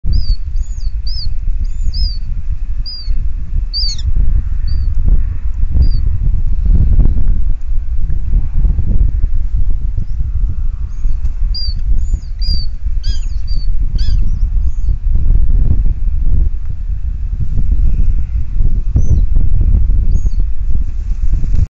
Milano real marcado con emisor-Webcam Eurokite
Esta webcam se ubica en el muladar de Las Pichillas (Binaced, Huesca, noreste de España) y es uno de los muladares controlados más antiguos de España. Desde 1980 está gestionado por el Fondo de Amigos del Buitre (FAB) y desde entonces llevan 40 años alimentando y censando esta población de aves.

LIFE EUROKITE

La instalación de esta cámara web en el muladar de Las Pichillas ha sido posible gracias al proyecto LIFE EUROKITE (LIFE18 NAT/AT/000048). Con el fin de conservar al milano real en toda Europa, en 2020 se ha comenzado este proyecto de conservación transfronterizo del milano real y otras especies de rapaces que se encuentran amenazadas en Europa. Este proyecto se llevará a cabo durante los próximos siete años en 26 países europeos con el apoyo de numerosos socios beneficiarios y cofinanciadores. La coordinación del LIFE depende de la organización austriaca MEGEG (Mitteleuropäische Gesellschaft zur Erhaltung der Greifvögel, Sociedad Centroeuropea para la Protección de las Rapaces) y SEO/BirdLife es uno de los socios beneficiarios en España.

Esta cámara web forma parte de una de las tres cámaras de que dispondrá el proyecto LIFE EUROKITE dentro de sus acciones de sensibilización pública y difusión de resultados. La acción se ha podido llevar a cabo gracias al apoyo del Fondo de Amigos del Buitre, el Ayuntamiento de Binaced y el Servicio Provincial de Huesca de la Diputación General de Aragón.